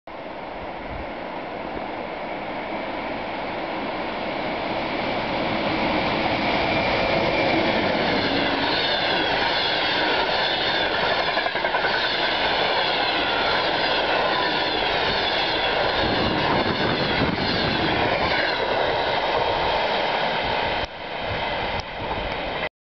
Russian express train at Kouvola, Finland part 2
Express train 501 from Kolari to Moscow.

Venäläisten tilausjuna numero 501 kuljettaa lomalaisia kotiin Kolarista.